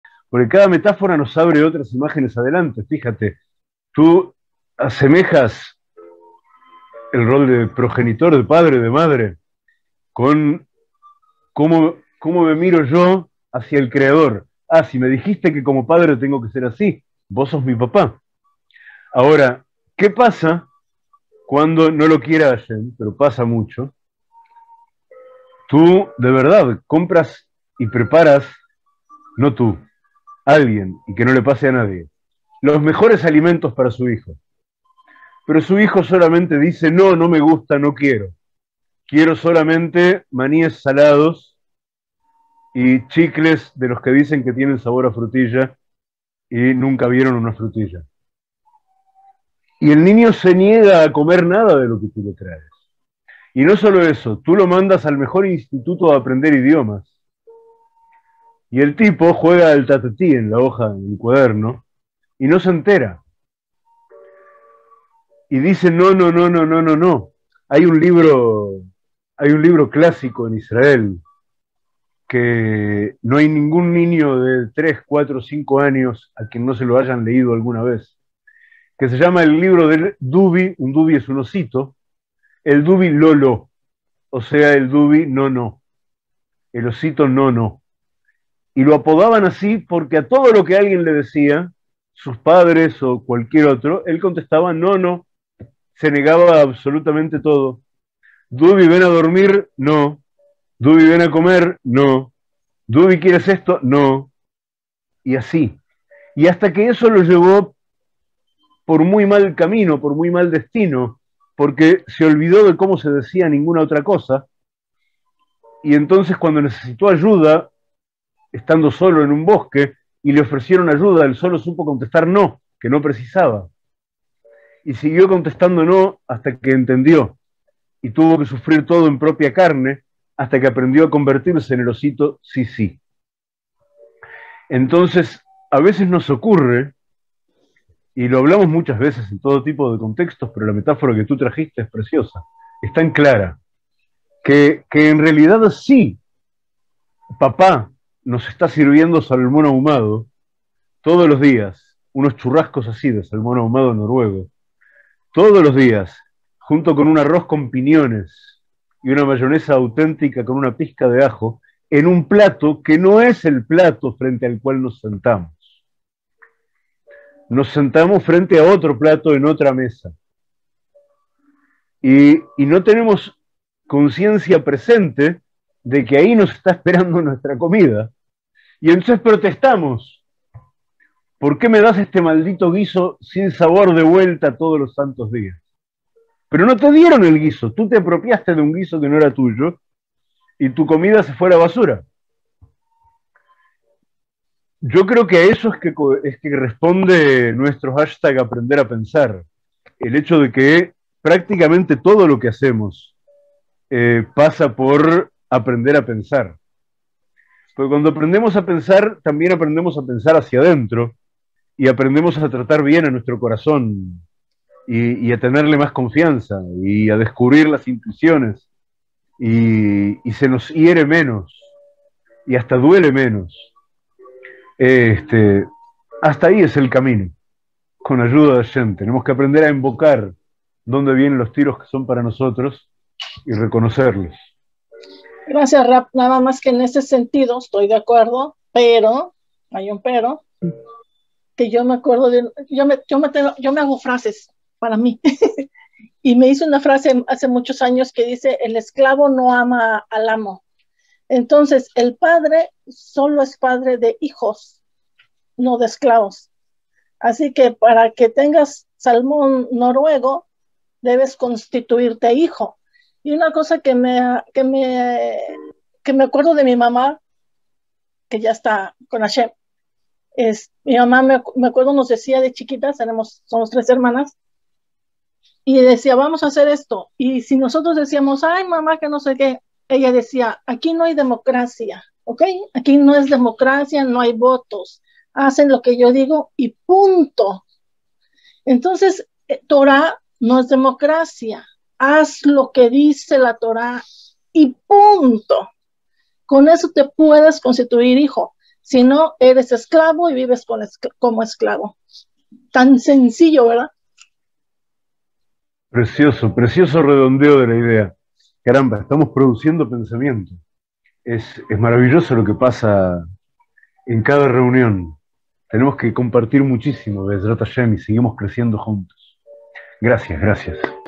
Te sirvieron manjares pero tú estabas comiendo porquería en otro lado
Fragmento del encuentro en que inauguramos el Fuego Perpetuo del Templo de Pasos en parashát Nasó, víspera de Shavuót.
Ven, forma parte de todo ésto maravilloso que está teniendo lugar en el Templo constituido de los Pasos de todos.

Te esperamos en el Templo de Pasos, donde estamos siempre celebrando.
Por todas partes, mientras se pueda: @danielginerman